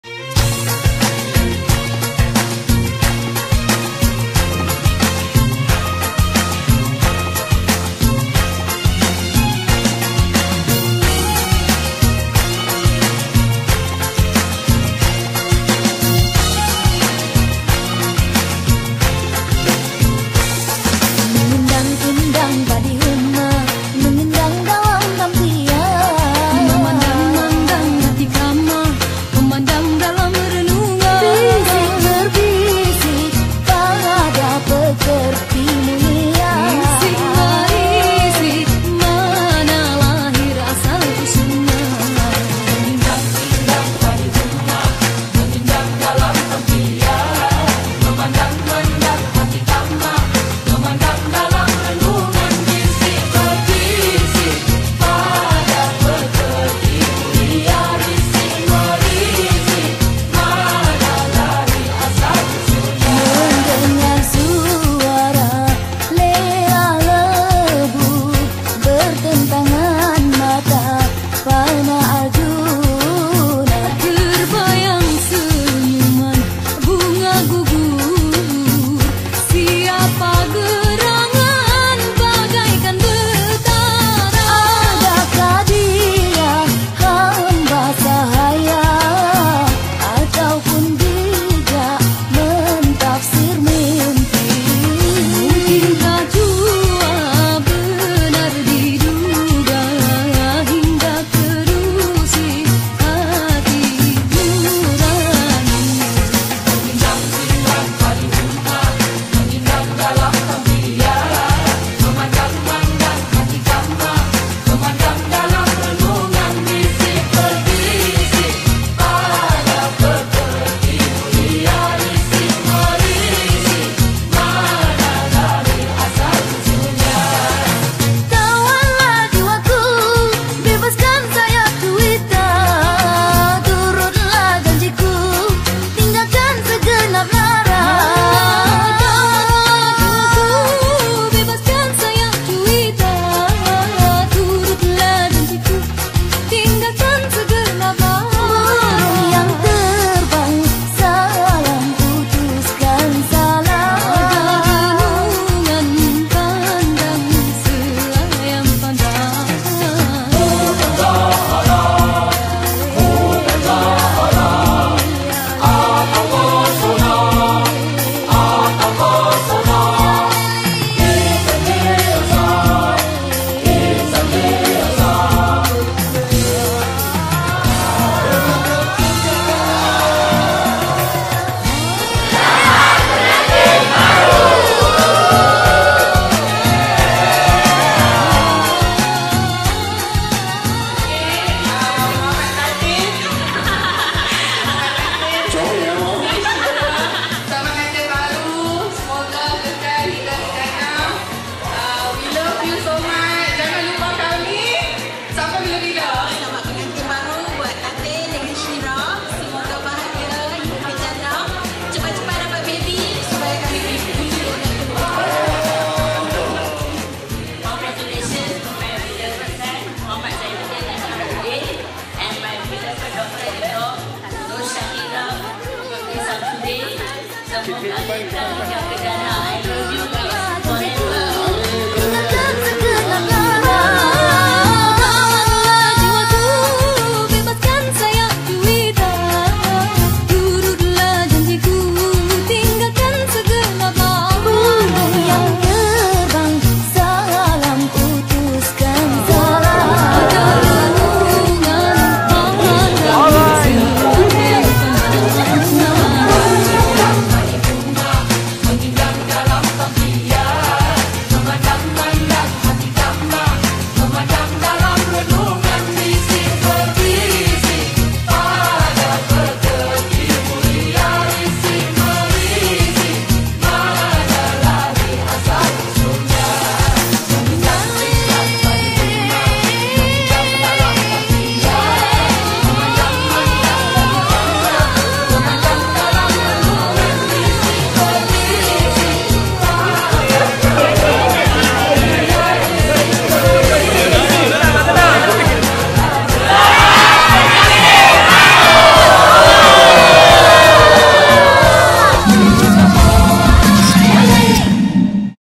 Zain & Shira | Majlis Persandingan | Malaysia Wedding | Indian Muslim Wedding
Video by; LangitTerbang